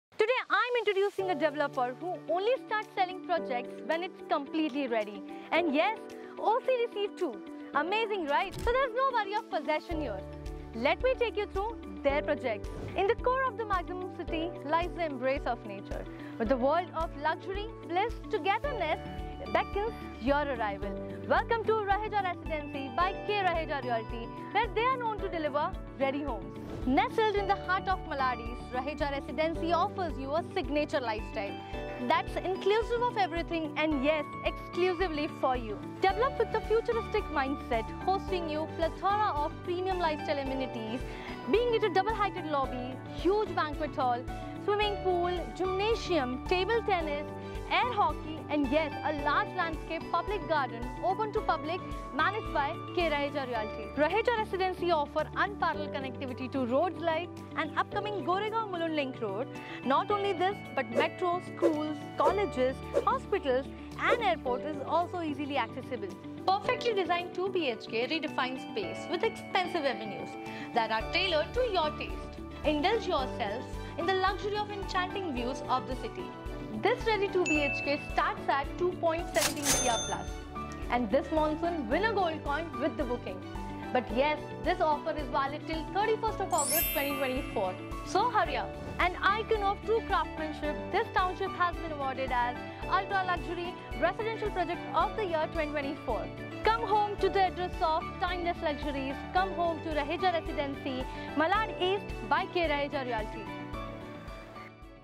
Welcome to Raheja Residency
Welcome to Raheja Residency – where your dream home becomes reality! 🌟 Located in the vibrant Malad East, our 2 BHK apartments offer unmatched convenience and comfort. Priced at Rs 2.17 cr+, it’s time to make the ultimate move. 🏠